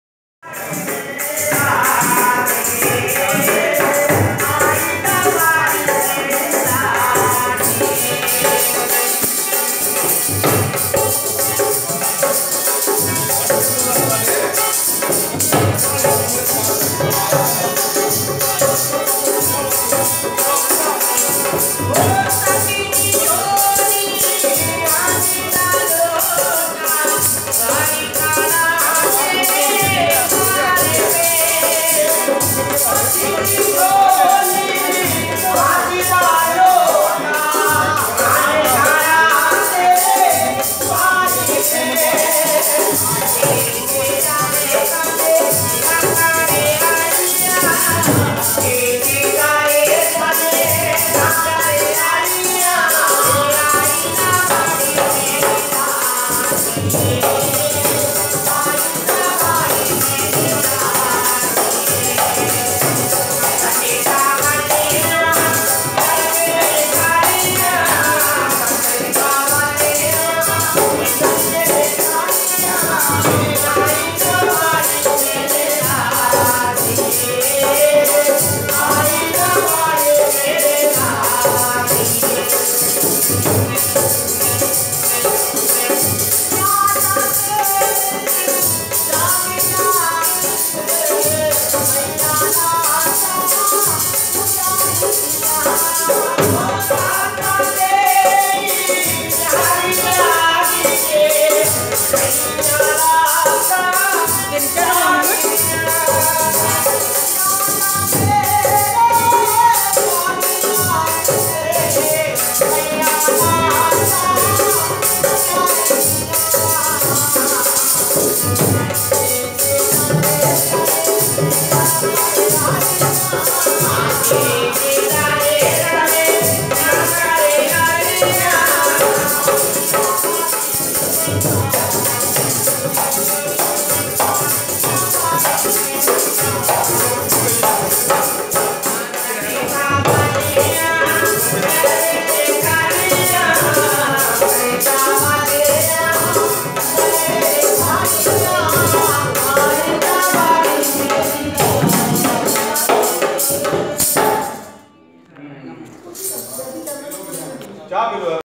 TALENT OF INDIA DURING LIVE PERFORMANCE AT CHANDIGARH | PART 2
Sants are performing live in early morning. Chandigarh residents see them here and there singing Bhajans in search of some snacks and food for them... Best ever high quality (royality free) video uploaded on youtube.